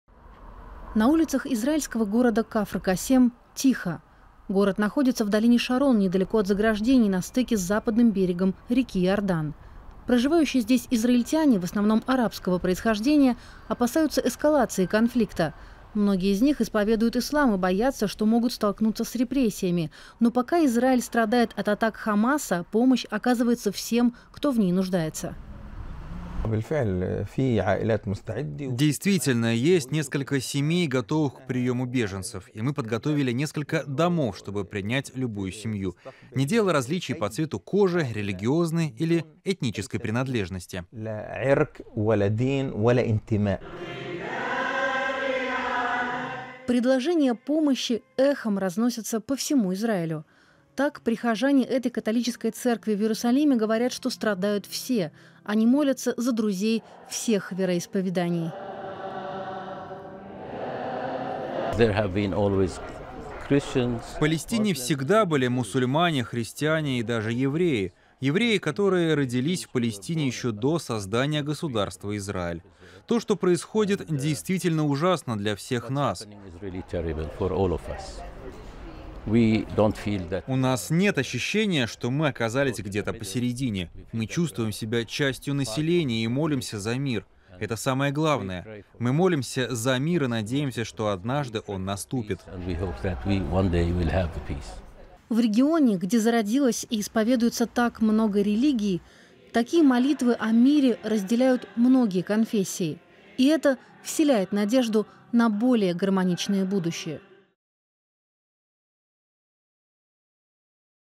Израильтяне всех конфессий и национальностей молятся за единство и мир
Пока израильская армия готовится к наземному наступлению на сектор Газа, многие граждане призывают к миру и единству. Израильские католики собрались на молитву за прекращение боевых действий, а израильские арабы открывают свои дома для нуждающихся семей.